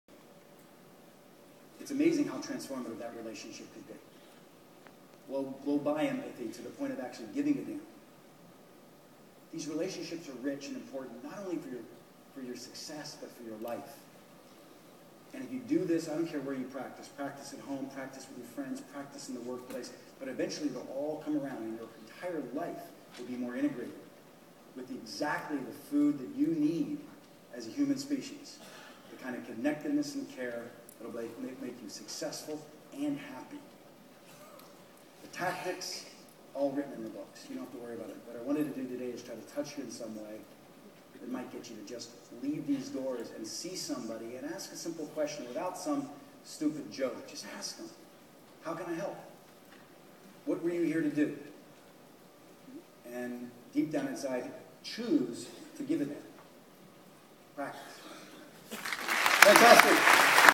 Keith Ferrazzi on relationships in life and business
3 PRD agents got the privilege of seeing Keith Ferrazzi today at AREC (Australian Real Estate Conference)
This is just a short excerpt from the end of his speech.

Getting to training relating to our business is so critical, we got a lot out of this session.

Sorry this clip so short, just learning to upload direct from iPhone.

Strongly recommend you get this guy's book.

Regards Mark Kentwell -Director